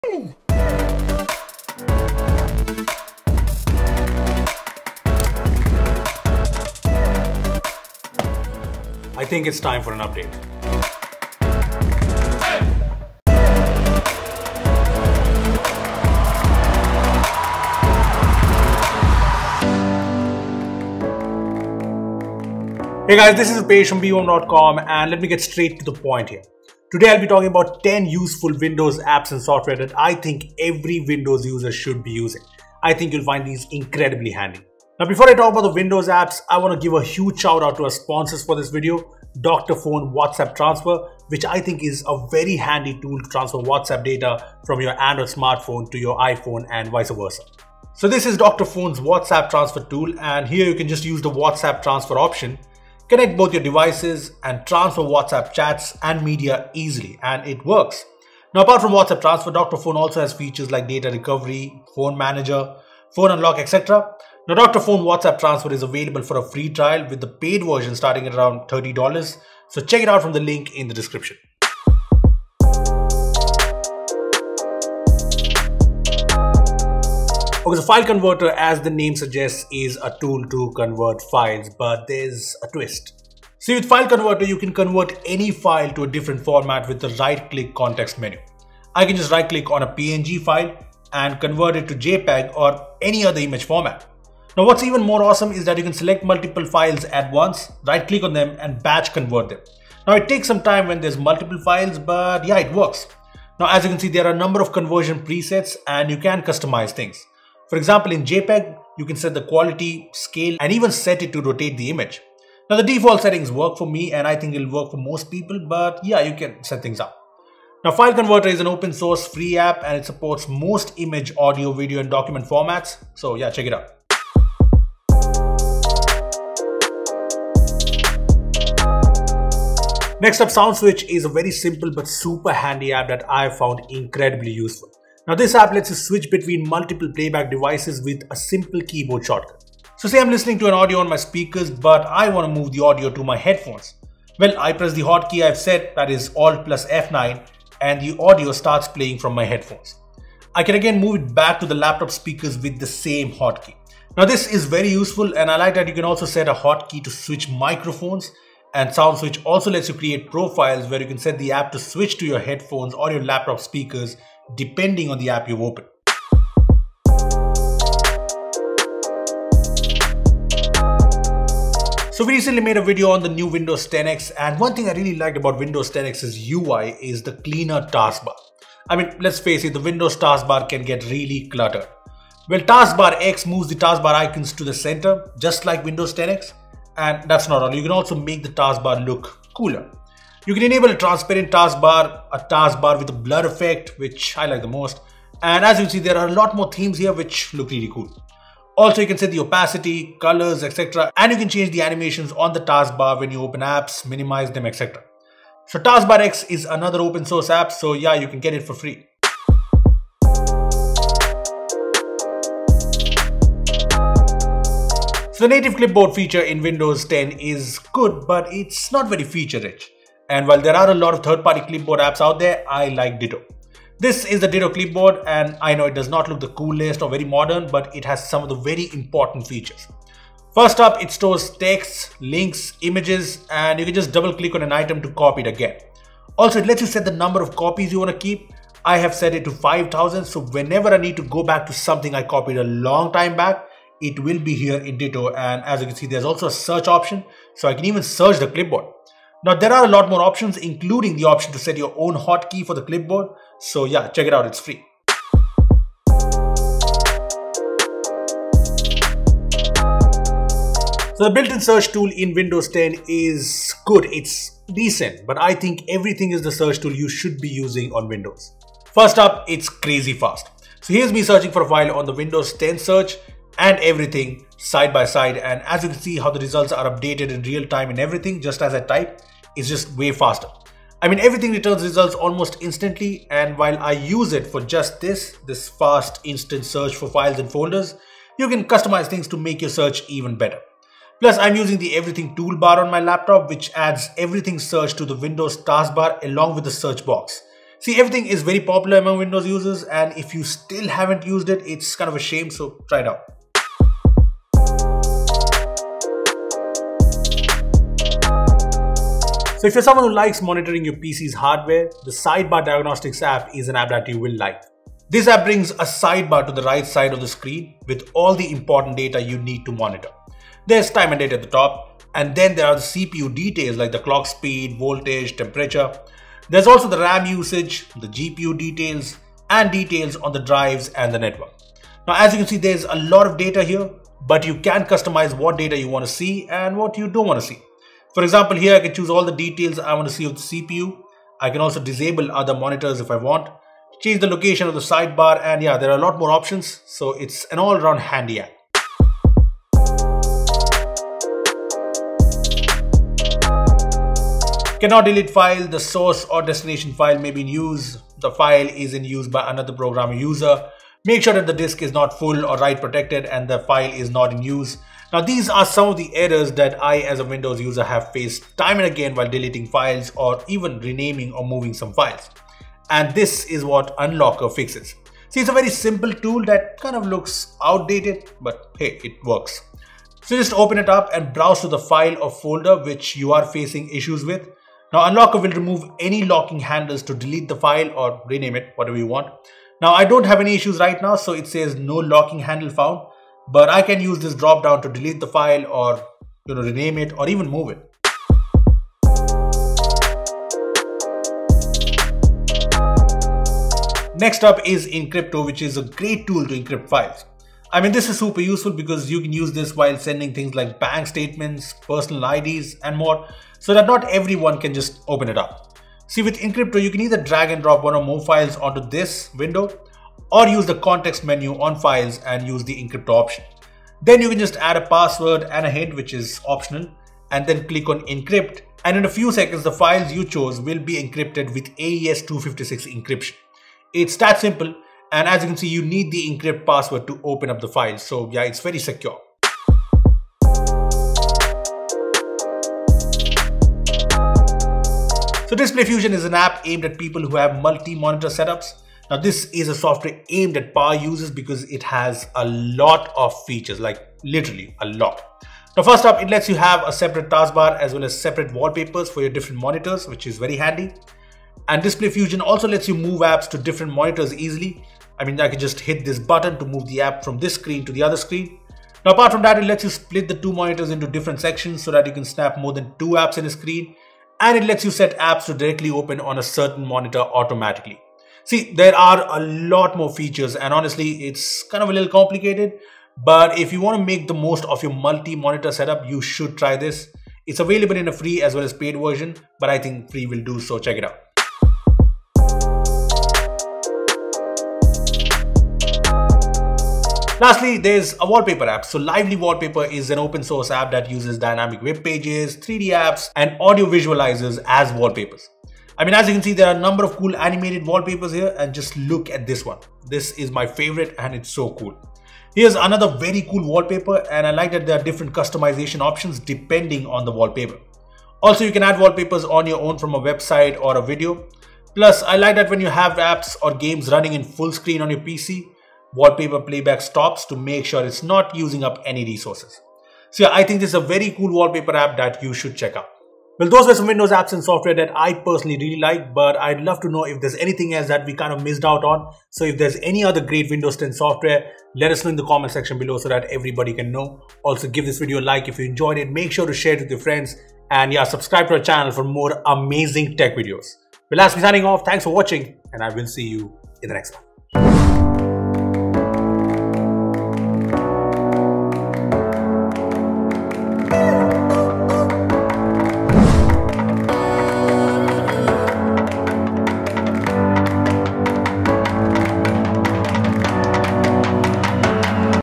10 Useful Windows Apps & Software to Use in 2021!
It's been since we talked about Windows apps and software, so here we are, talking about 10 useful Windows apps and software every Windows user should be using.